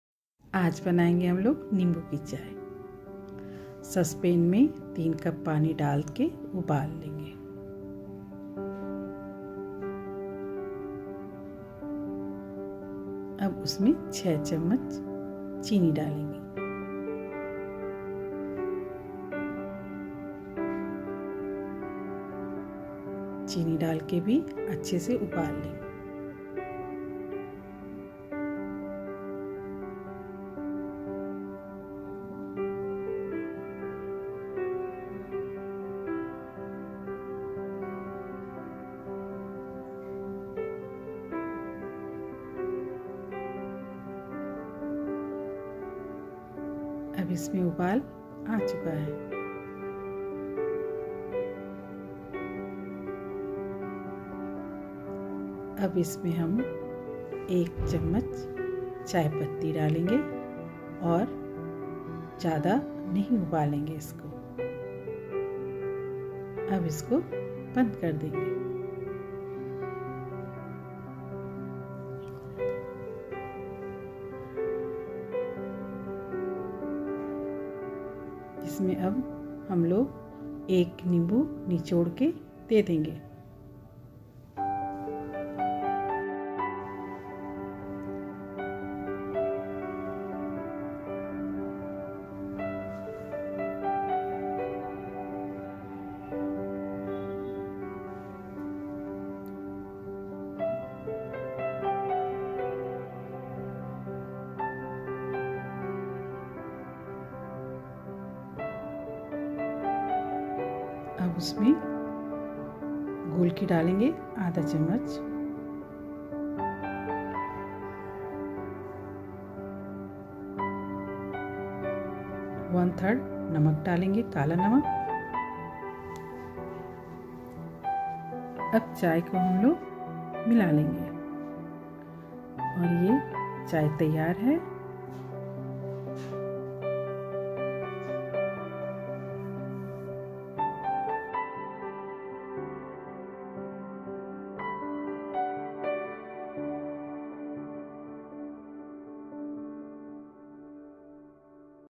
How to make lemon tea | नींबू की चाय | Nimbu Chai recipe
Lemon juice squeezed in black tea and mixed with some sugar and masalas - nimbu chai is more refreshing than ever.

Ingredients:
3cups water
1lemon's juice
6tbsp sugar
1tbsp tea leaves
1/3tbsp black salt (Kala namak)
1/2tbsp black paper